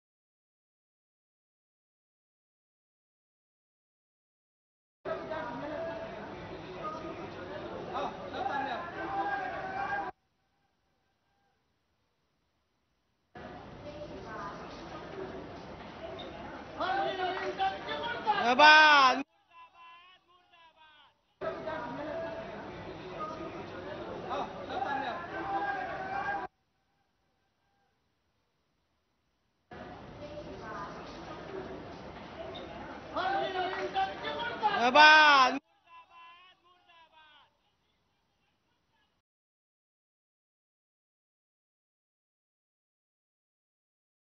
Watch: Ink attack on Indian National Trade Union Congress President Mahabal Mishra - ANI News
Bilaspur (Chhattisgarh), July 09 (ANI): In a shocking incident captured on camera, a group of miscreants can be seen pouring black ink on Indian National Trade Union Congress (INTUC) President Mahabal Mishra at railway station in Chhattisgarh's Bilaspur.